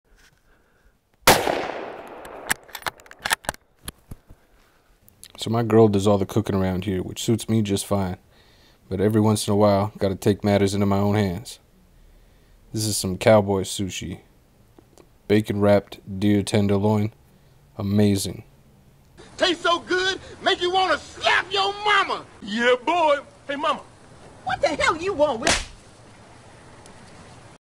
Cowboy Sushi
Deer tenderloin wrapped in bacon. Venison is delicious. Simple recipe.